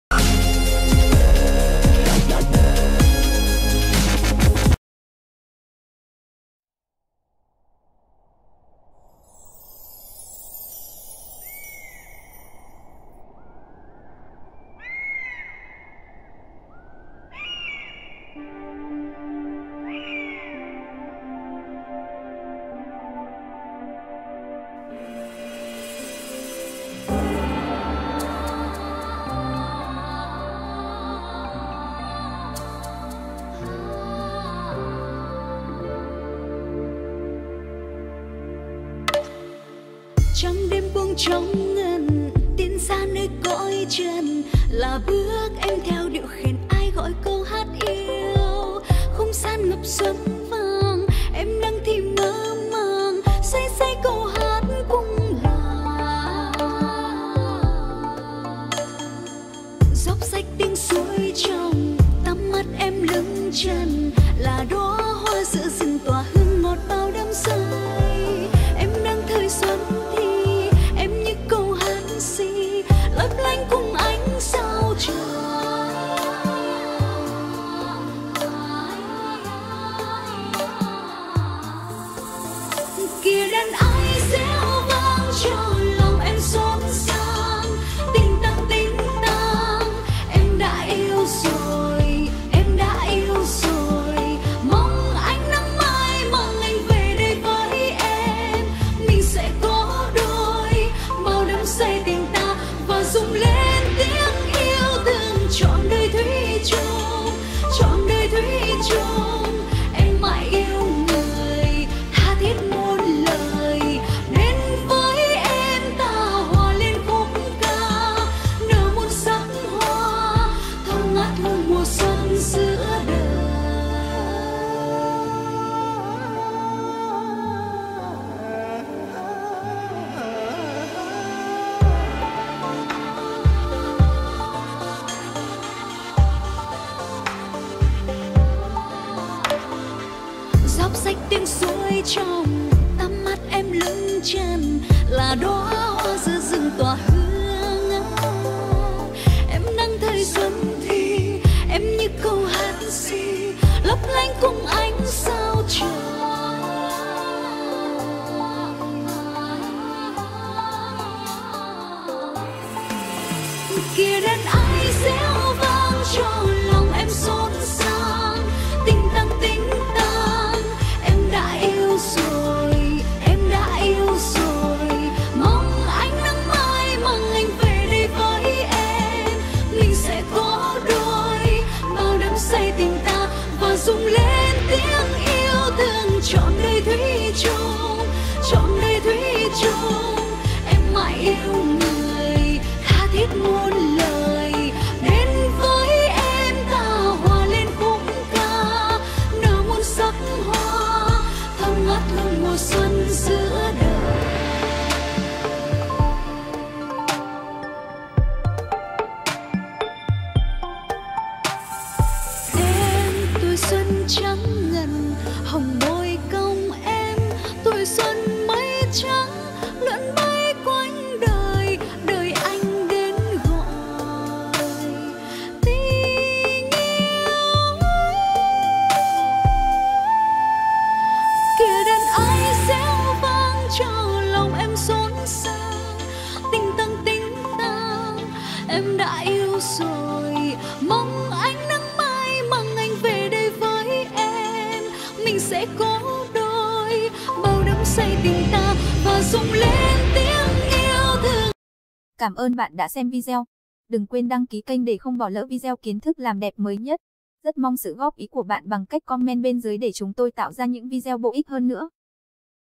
TRỘN DẦU DỪA Với Thứ này rồi ĐẮP LÊN MẶT Da CĂNG MƯỚT Đẹp Đến Chóng Mặt Luôn
-  99% phụ nữ “điên cuồng” đi mua oxy già về trị mụn, dưỡng da, làm trắng răng sau khi phát hiện công dụng: